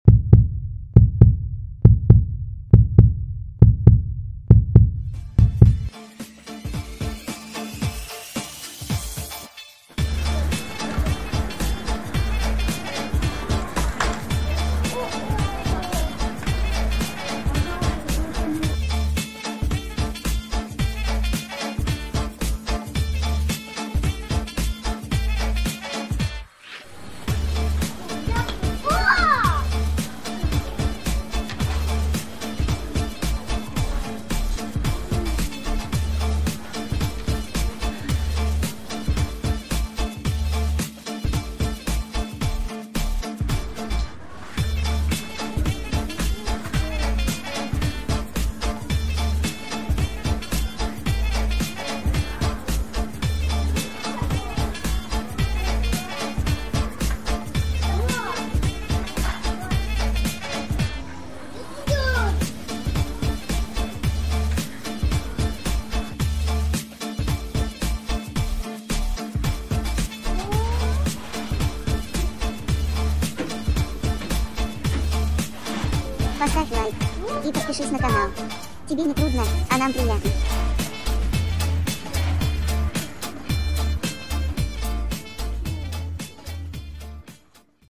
Мишутка инженер. Mishutka is an engineer
Мишутка попал в техно центр где большой ассортимент разных конструкторов на базе лего, где с огромным удовольствием собирал лего и играл.       Mishutka got into a techno center where there is a large assortment of different lego-based designers, where he collected legos and played with great pleasure.